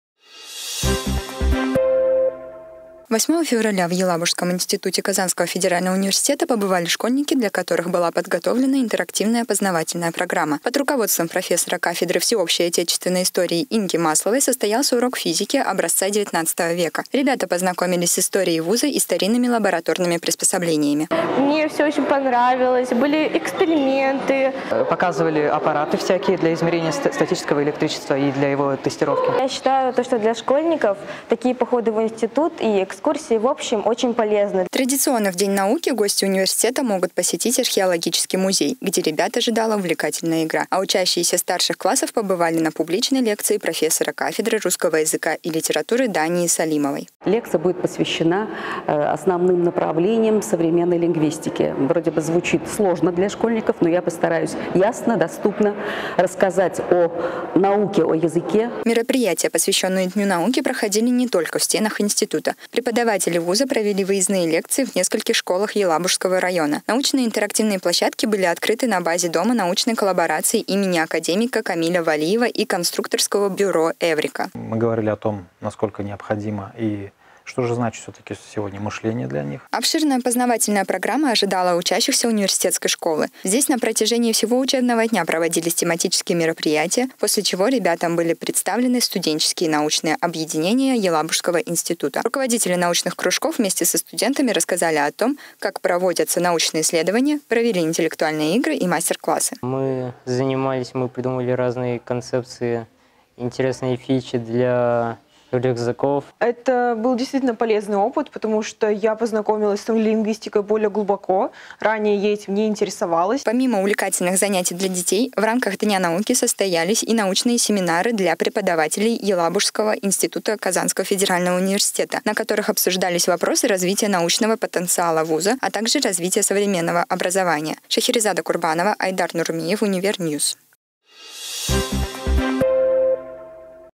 ПРАЗДНОВАНИЕ ДНЯ НАУКИ В ЕЛАБУГЕ
В Елабужском институте КФУ стартовала неделя, посвященная празднованию Дня российской науки. Подробности в нашем сюжете.